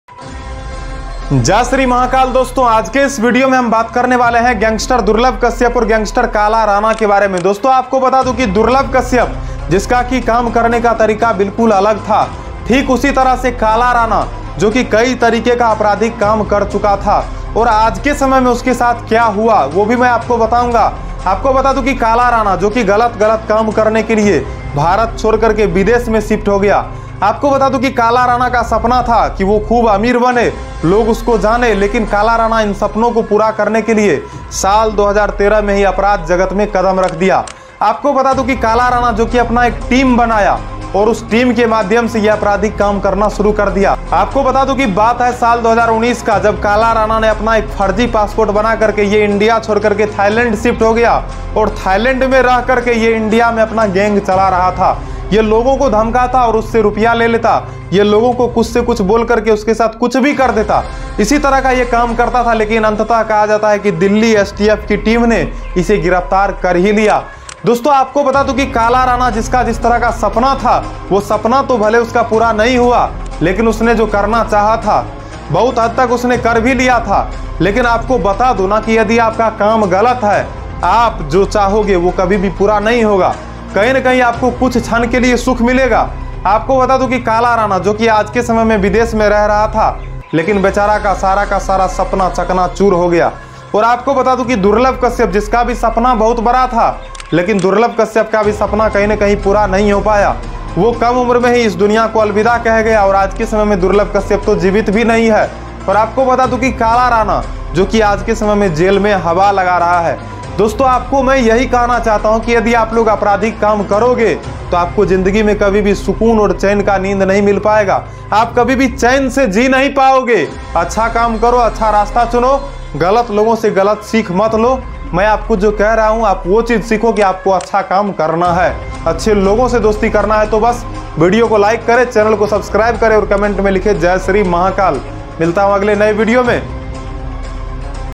दुर्लभ कश्यप और काला राणा का कहानी ! | Kala Rana Story With Durlabh Kashyap | Explained By Sarkar
This video is not to spread negativity so be aware and learn good things

I never support any gangsters I love Indian Fouji 🇮🇳

Lots of ❤️